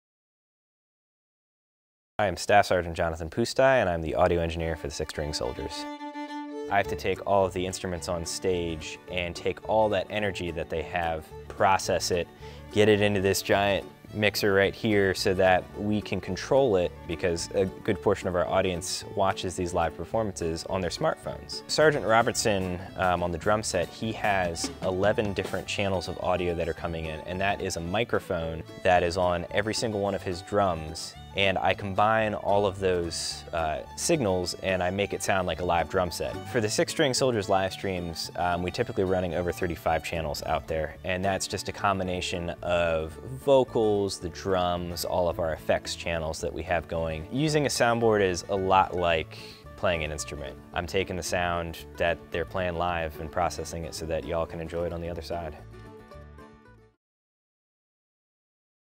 Behind the Mixing Board | Staff Sgt. Jonathan Pusztai
Ever wonder how such a high quality sound makes it from our Soldiers to your listening device? Meet the expert sound engineer that makes it all happen!

U.S Army video by Sgt. 1st Class Jimi Little

Public Affairs
The United States Army Field Band
4214 Field Band Drive, STE 5330